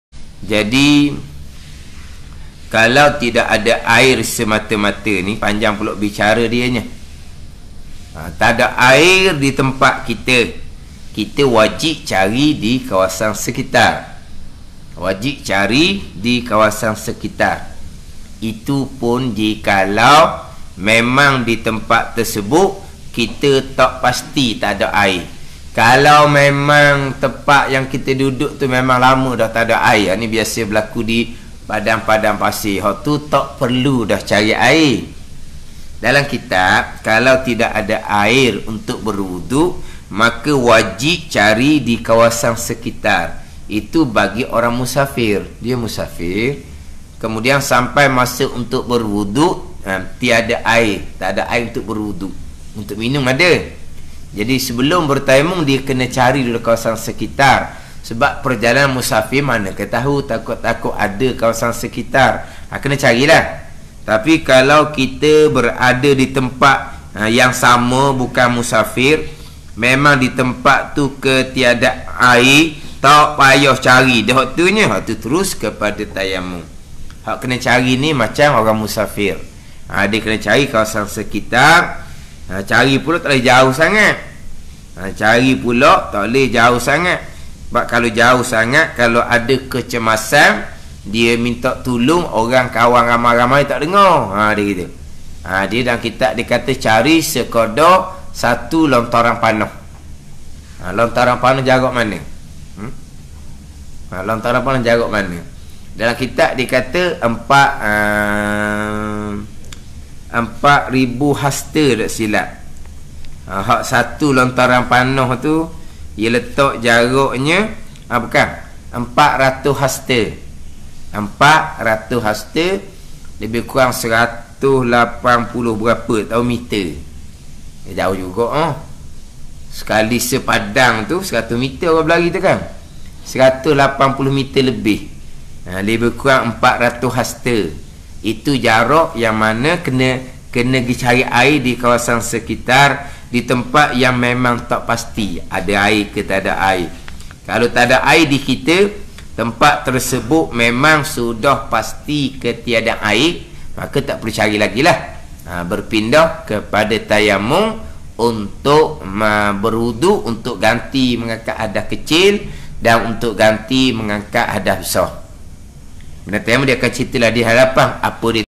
Bab menyatakan ketiadaan air
Bab tayammum(sebab pertama ketiadaan air semata-mata untuk wuduk dan mandi wajib) : Wajib cari dahulu air dengan kadar jarak 1 anak panah@400 hasta@180 meter lebih bagi orang musafir.